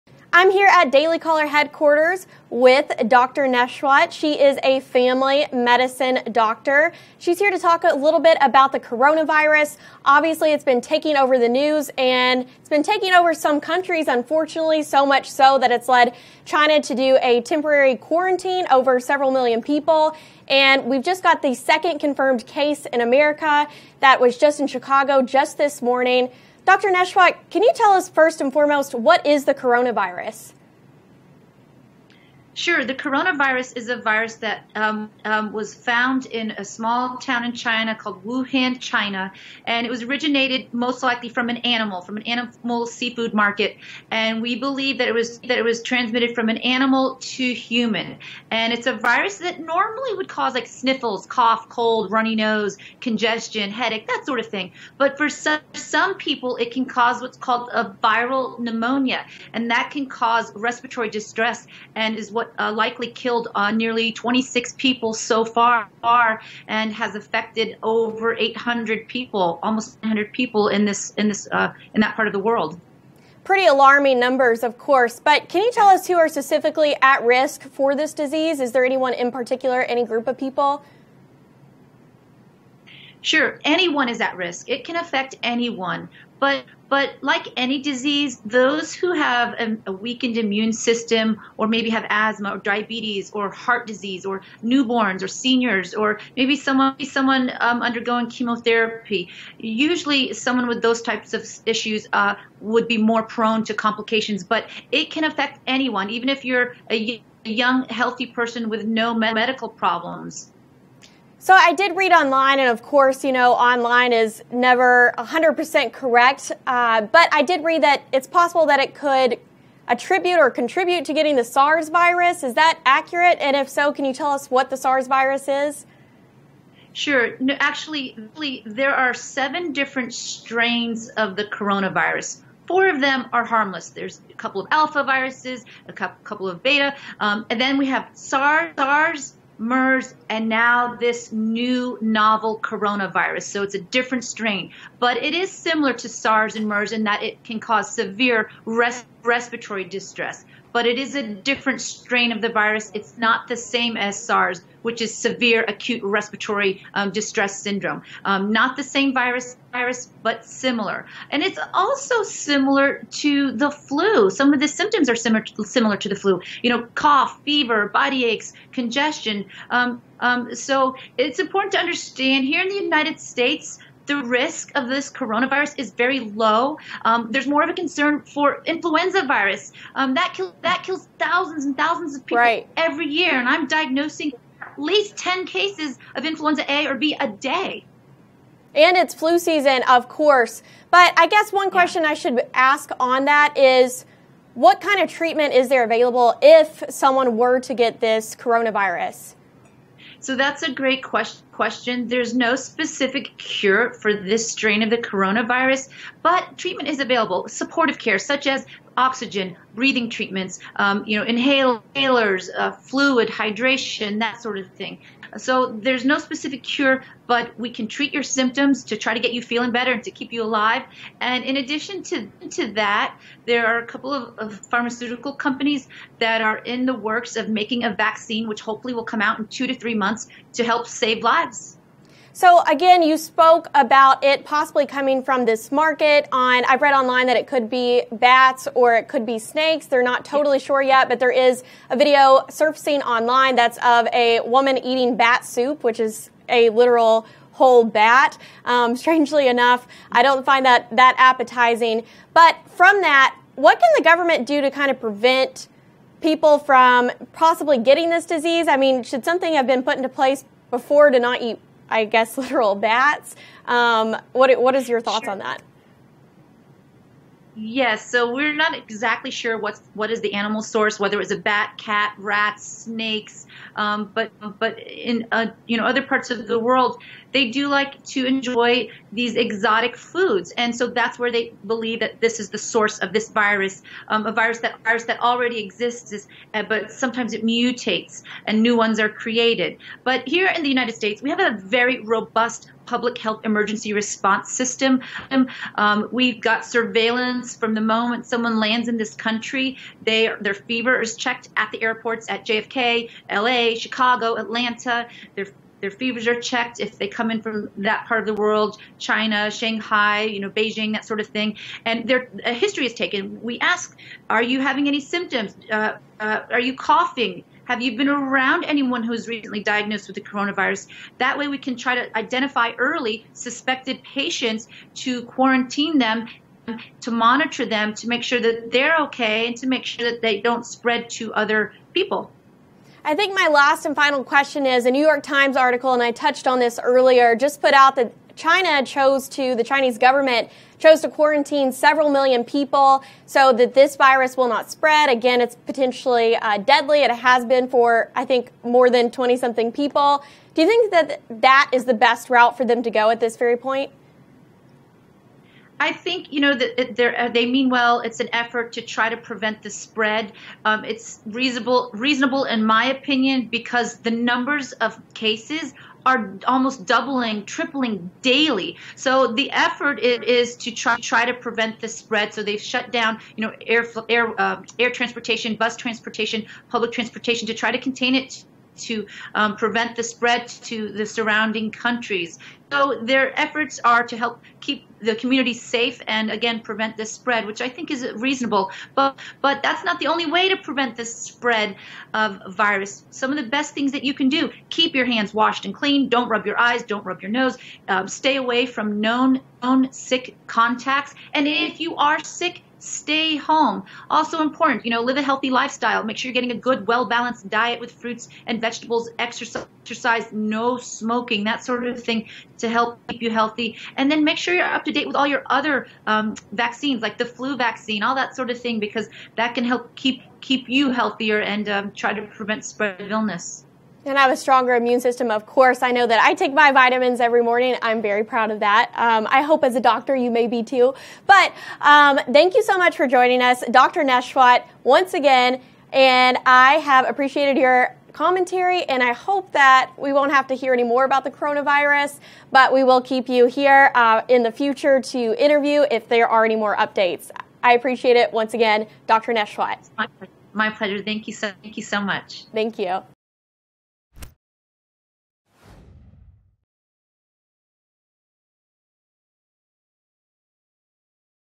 The Coronavirus: Dr. Nesheiwat Talks Protection, Prevention, and Government Intervention
Daily Caller Video Columnist Maranda Finney interviews Dr. Janette Nesheiwat, a family medicine doctor, on the coronavirus. Dr. Nesheiwat discusses with Maranda the necessary precautions one must take to maintain a healthy immune system in order to best prevent yourself from getting the potentially deadly coronavirus. They also discuss where the disease may have originally come from and the efficacy of the Chinese government's actions to curb the spread of the coronavirus.